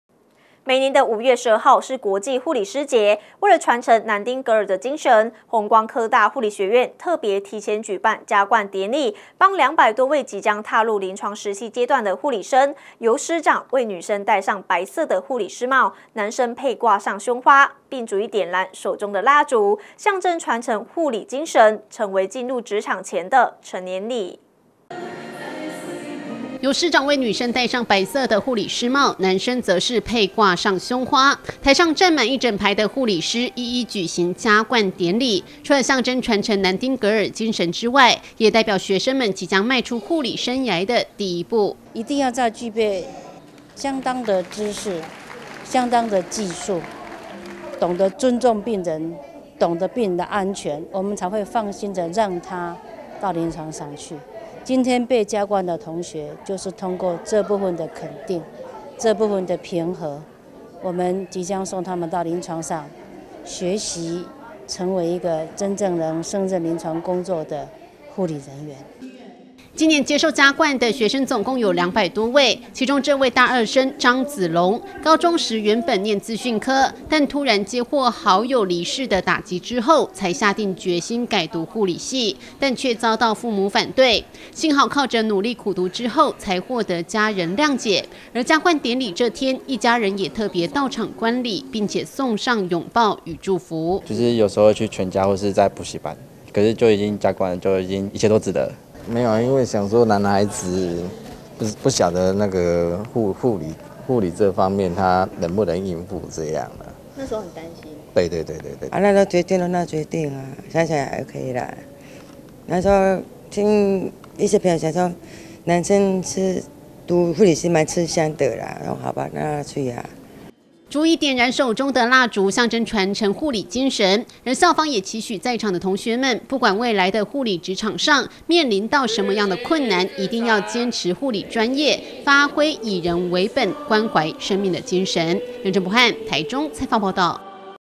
大台中新聞 沙鹿護理系辦加冠典禮
每年的5月12號是國際護師節，為傳承南丁格爾精神，弘光科大護理學院特別提前舉辦加冠典禮，幫2百多位即將踏入臨床實習階段的護理生，由師長為女生戴上白色的護理師帽、男生佩掛上胸花，並逐一點燃手中的蠟燭，象徵傳承護理精神，成為進入職場前的成年禮。